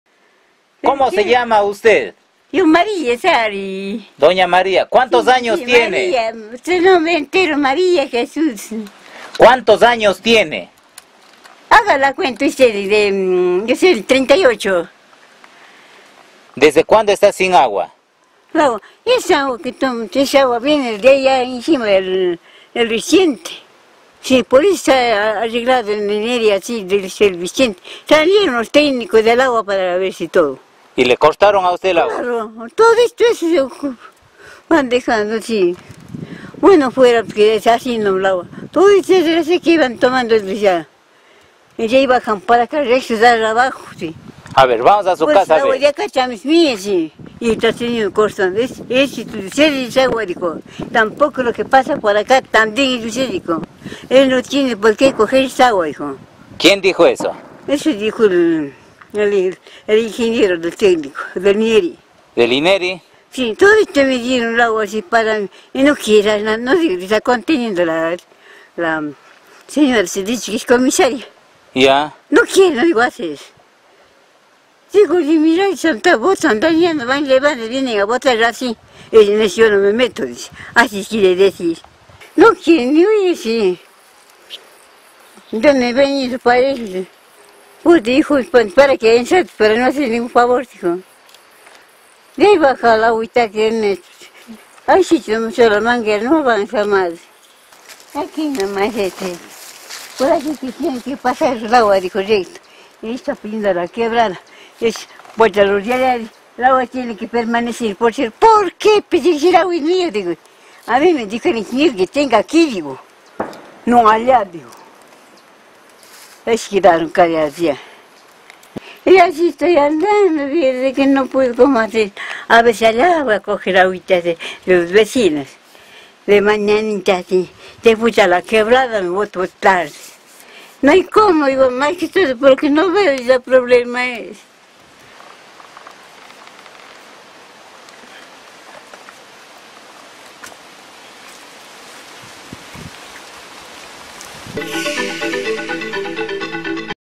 Anciana no tiene agua 5 meses en su casa
A la Sra. María Sari, es usual verle todos los días por Girón pidiendo caridad, es la única forma de subsistencia que tiene, desde que dejó de trabajar en su telar que aún lo conserva en su casa en el sector del Chorro.
A sus 75 años de edad tiene su memoria lucida pero es difícil comunicarse con ella porque le falta la vista y padece de sordera. Para llegar a su casa desde la vía al Chorro hay que caminar unos 100 metros por un angosto trecho que lo recorre con dificultad.
Desde hace cinco meses, recuerda que dejó de llegar el agua mediante una manguera a la acequia que queda a 10 metros de su domicilio, desde entonces tienen que ir a pedir a los vecinos o bajar al rio.
Según un morador del lugar, hace algunos meses, vecinos -entre ellos familiares de la perjudicada- realizaron trabajos en los terrenos por donde pasa la manguera y la inhabilitaron, la anciana dijo haber denunciado a las autoridades pero nadie le presta atención "¿para qué han entrado para no hacer ningún favor?" Web